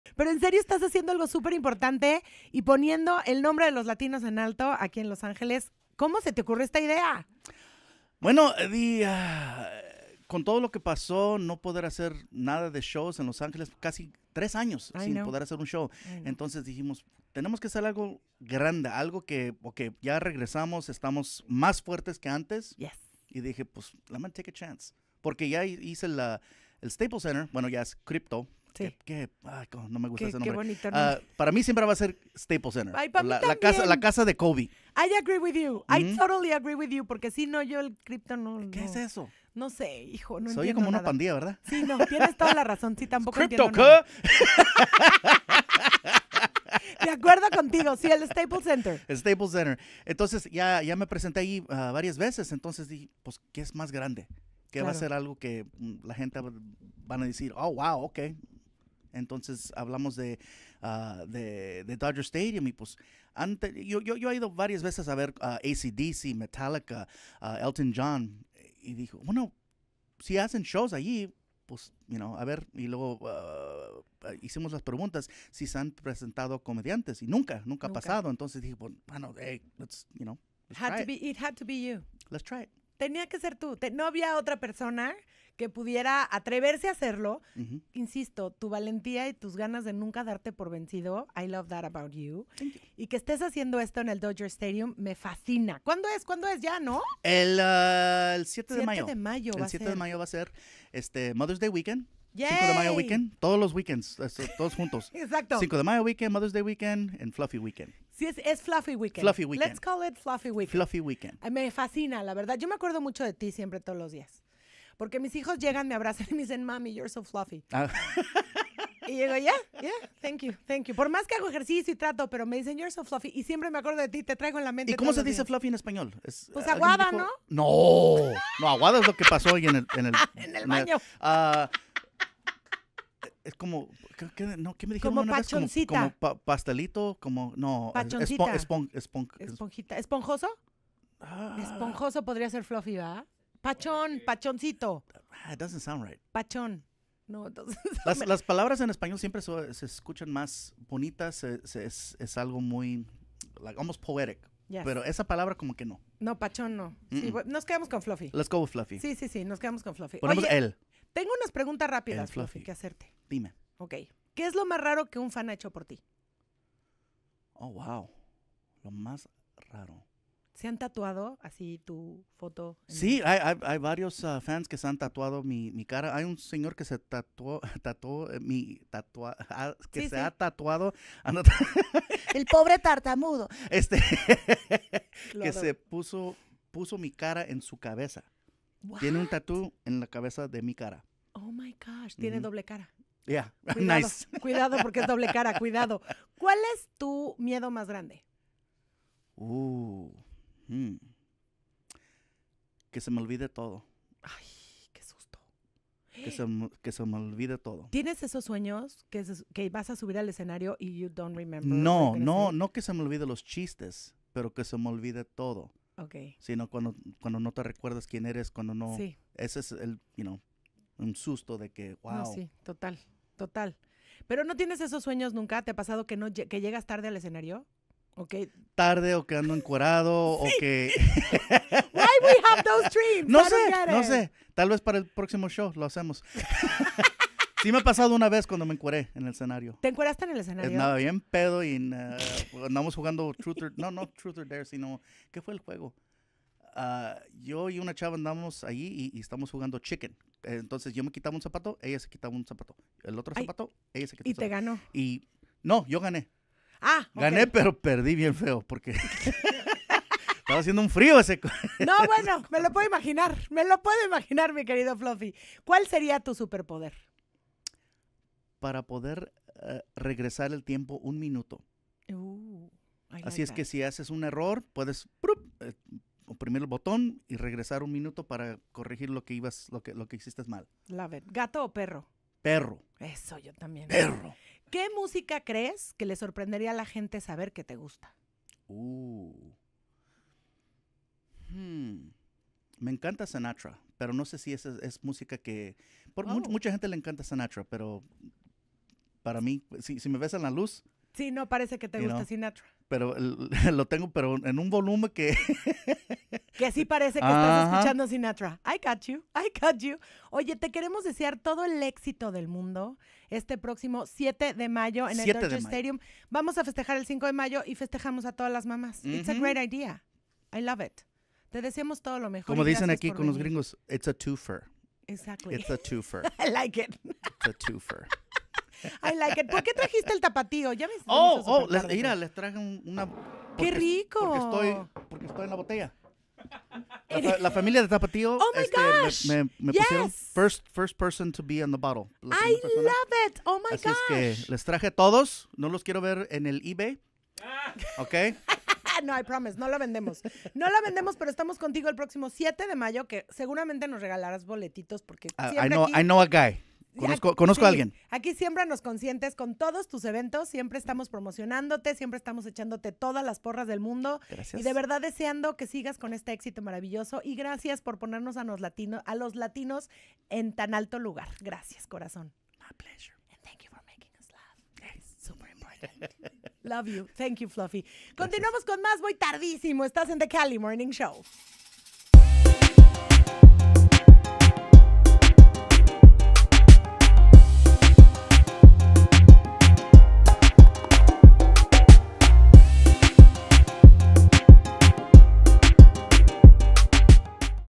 Gabriel Iglesias Will Make History At Dodger Stadium
Gabriel Iglesias llegó al estudio de Cali 93.9 y conversó con Angélica Vale sobre su show en el Dodger Stadium.

Gabriel Iglesias, comúnmente conocido como Fluffy, es un comediante y actor. I'm Not Fat... I'm Fluffy y Hot & Fluffy son dos de sus especiales de stand-up para cadenas de televisión como Comedy Central y Netflix.

Durante la entrevista, Gabriel habla con Angélica sobre mejorar su desempeño y lo que sigue para su carrera. El mencionó “Que es mas grande, que es algo que la gente va dicer oh wow ok! Entonces hablamos de Dodger Stadium.”

Gabriel es el primer comediante en actuar en Dodger Stadium. Para ser parte de esta gran historia de comedia, no te pierdas a Gabriel Iglesias el 7 de mayo en Dodger Stadium.